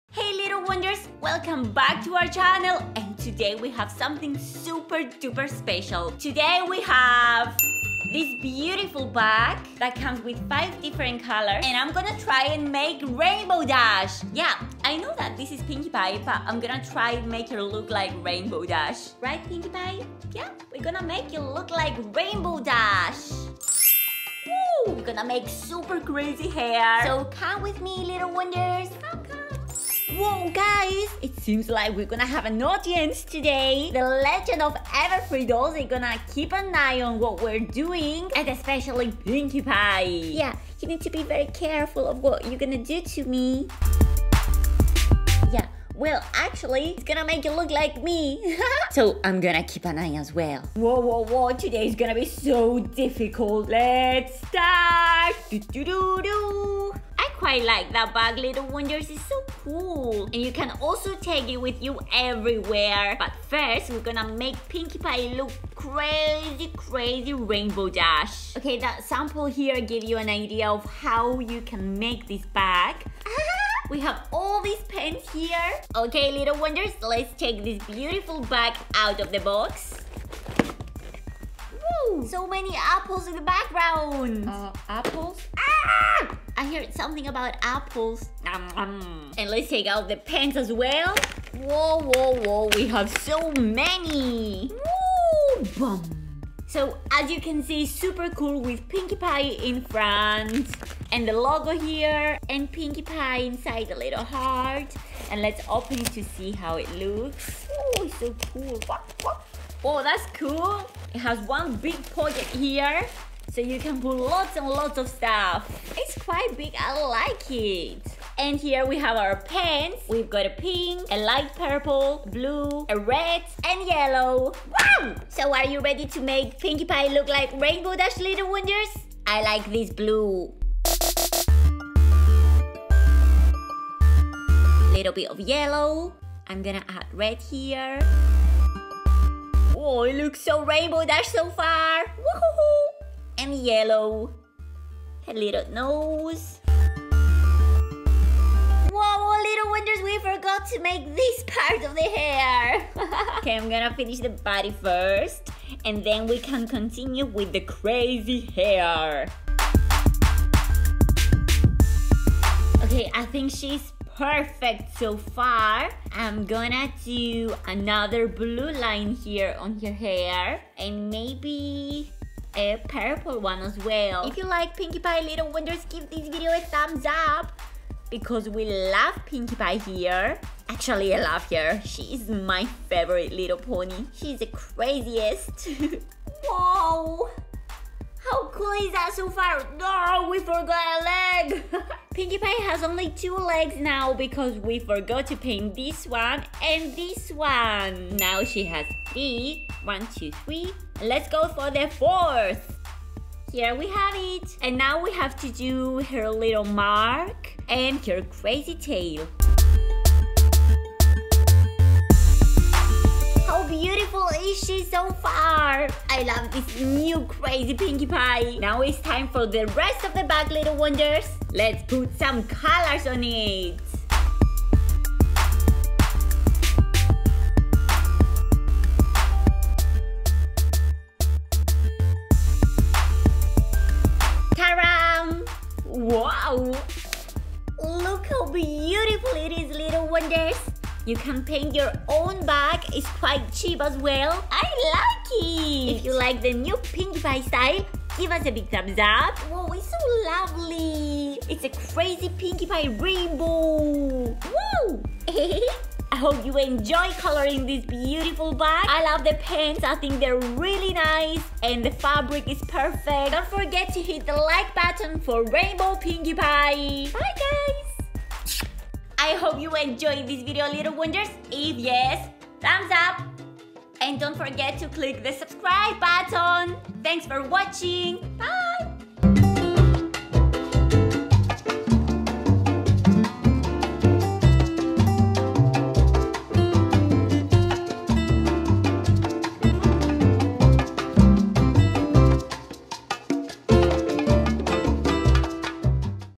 Scribble Me Pinkie Pie Colour In Messenger Bag - DIY Paint Pinkie Rainbow Dash!!!!
💖 Hey guys, Today we are gonna be painting yayaya :D We have this awesome MLP messenger bag, that comes with pinkie pie on it, BUT we are going to paint pinkie pie as Rainbow Dash!! Haha is gonna be so much fun so jump in and check how Pinkie pie looks like as Rainbow Dash ;) Don't forget to subscribe to our channel guys, and give this video a thumbs up please💖 Love youuuu!